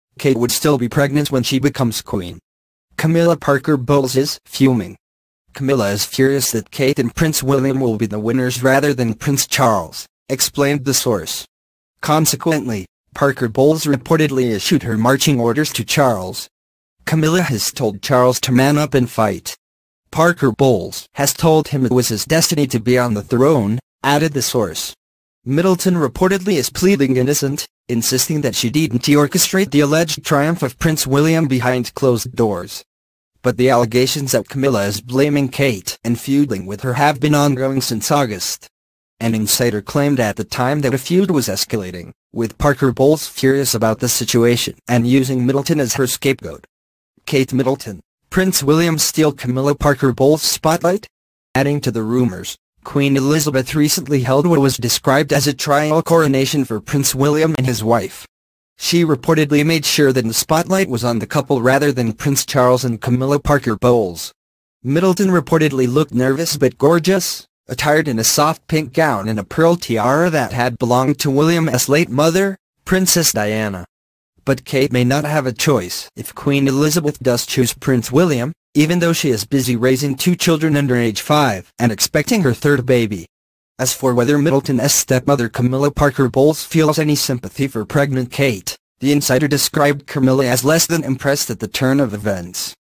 “Kate would still be pregnant when she becomes Queen Camilla Parker Bowles is fuming ”
Camilla is furious that Kate and Prince William will be the winners rather than Prince Charles, explained the source. Consequently, Parker Bowles reportedly issued her marching orders to Charles.

“Camilla has told Charles to ‘man up and fight.’ [Parker Bowles] has told him it was his destiny to be on the throne,” added the source.
Middleton reportedly is pleading innocent, insisting that she didn’t orchestrate the alleged triumph of Prince William behind closed doors. But the allegations that Camilla is blaming Kate and feuding with her have been ongoing since August. An insider claimed at the time that a feud was escalating, with Parker Bowles furious about the situation and using Middleton as her scapegoat.

Kate Middleton, Prince William Steal Camilla Parker Bowles’ Spotlight?
Adding to the rumors, Queen Elizabeth recently held what was described as a “trial coronation” for Prince William and his wife. She reportedly made sure that the spotlight was on the couple rather than Prince Charles and Camilla Parker Bowles.

Middleton reportedly looked “nervous” but gorgeous, attired in a soft pink gown and a pearl tiara that had belonged to William’s late mother, Princess Diana. But Kate may not have a choice if Queen Elizabeth does choose Prince William, even though she is busy raising two children under age five and expecting her third baby.

As for whether Middleton’s stepmother Camilla Parker Bowles feels any sympathy for pregnant Kate, the insider described Camilla as “less than impressed at the turn of events.”